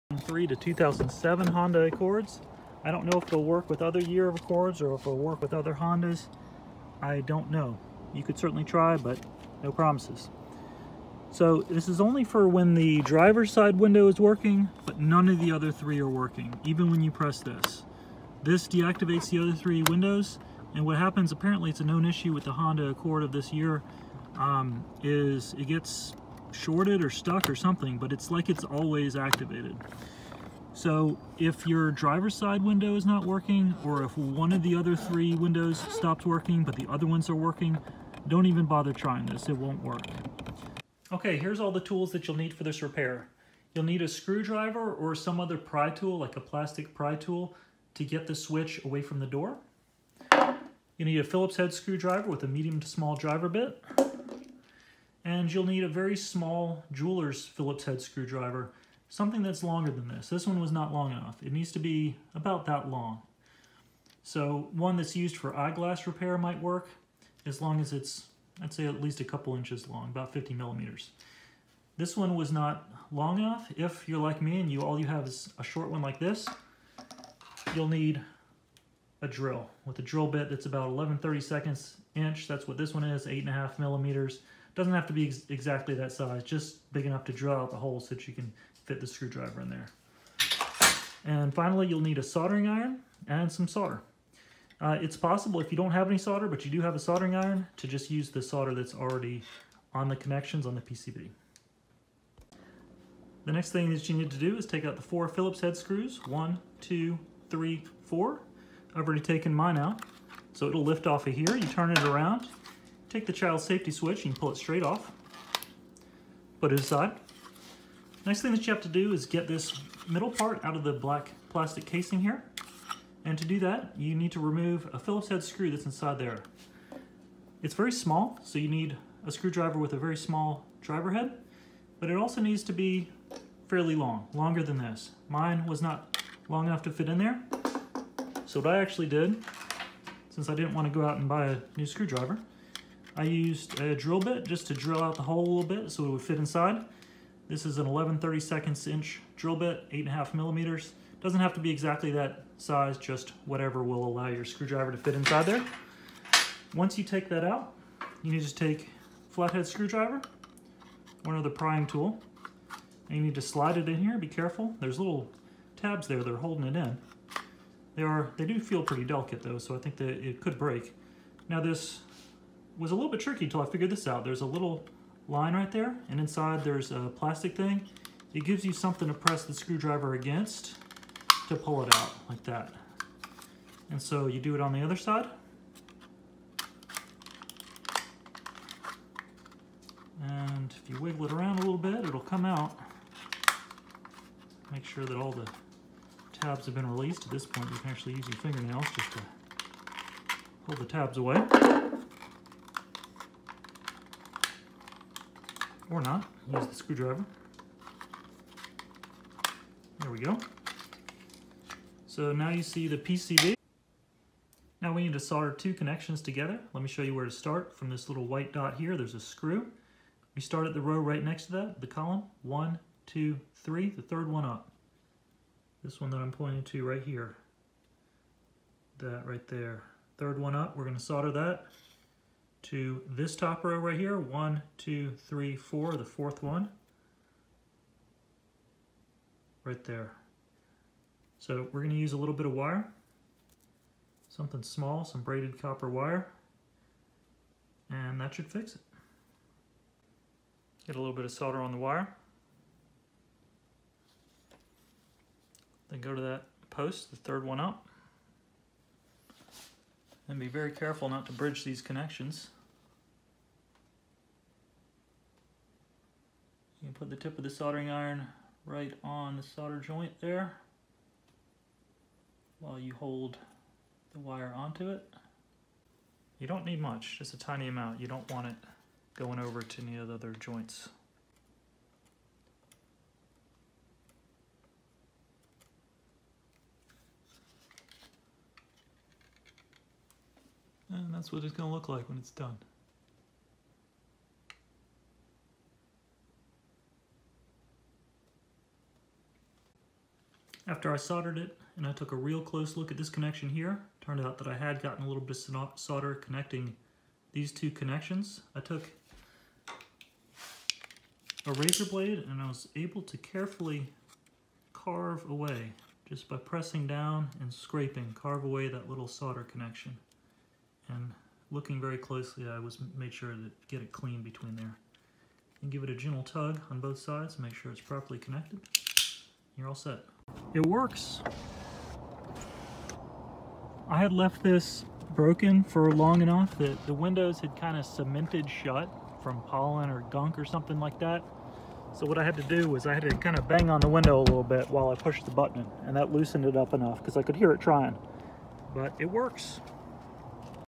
Fix Honda Accord 2003-2007 window switch when passenger and rear windows not working
This will work for Honda Accord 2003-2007. I don't know if it will work on other Honda models or years. This will ONLY work if the driver side window works, but the passenger side and rear windows have stopped working. It requires tools but no parts.